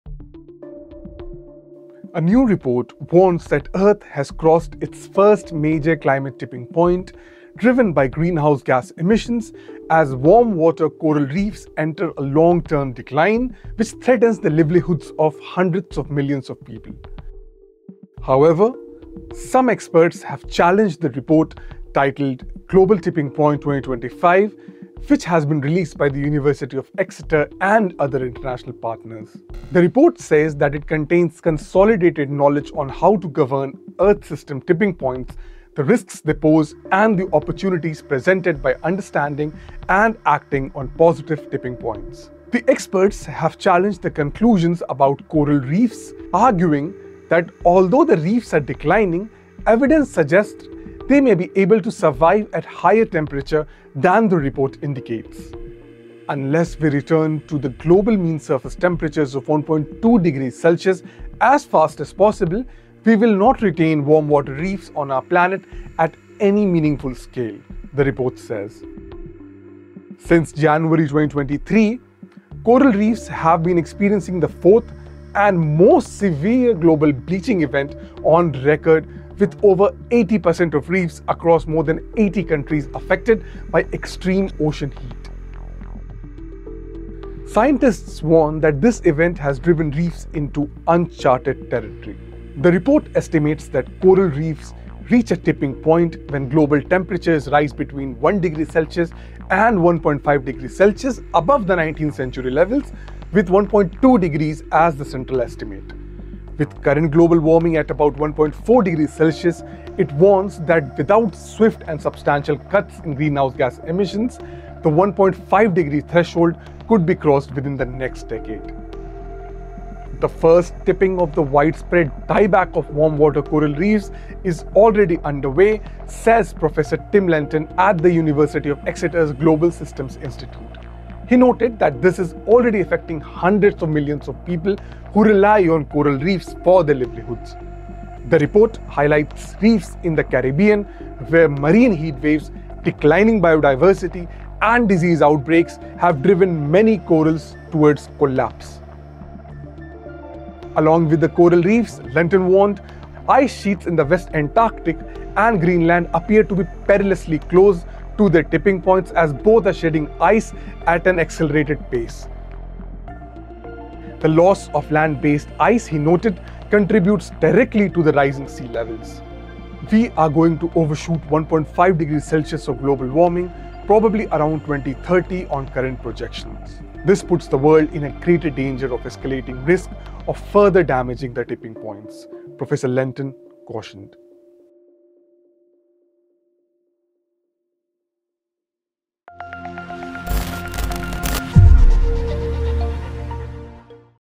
Planet Earth 1st Tipping point | Coral Reefs’ Long-Term Decline
A new report warns that Earth has crossed its first major climate tipping point as warm-water coral reefs enter long-term decline, threatening millions of livelihoods.
Scientists say reefs are nearing collapse amid record ocean heat and global bleaching events, while some experts argue they may be more resilient than feared.